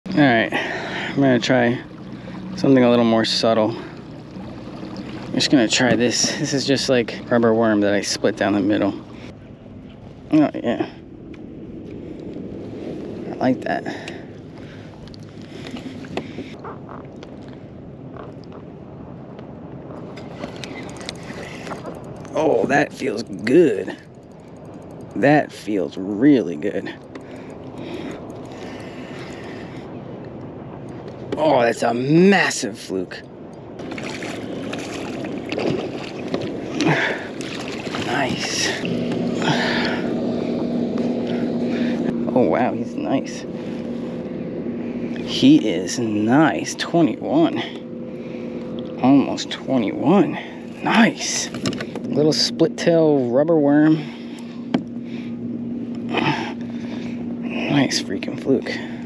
A fluke (summer flounder) on a bass jig worm
Sometimes you have to switch up the profile as the usual bucktail with gulp grub was only getting me sea robins